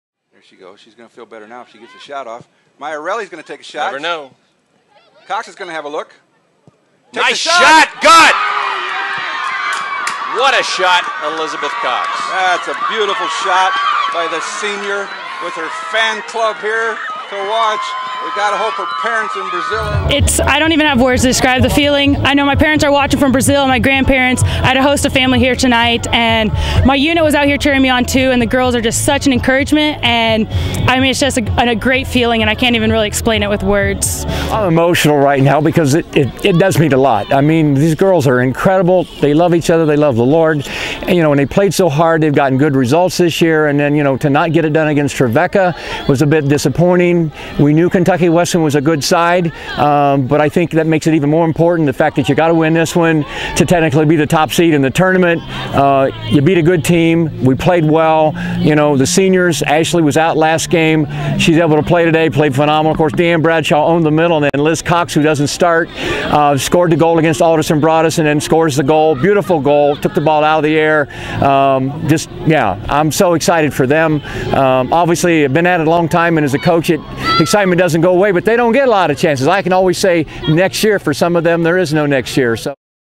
Women's Soccer vs KY Wesleyan, 10/26/13
Senior forward Elizabeth Cox scored in the 68th minute and the Lady Jackets defeated Kentucky Wesleyan, 1-0, in the regular season finale on Senior Night. CU secured the #1 seed in the upcoming G-MAC Tournament. Cox and Head Coach John McGillivray talk about the win.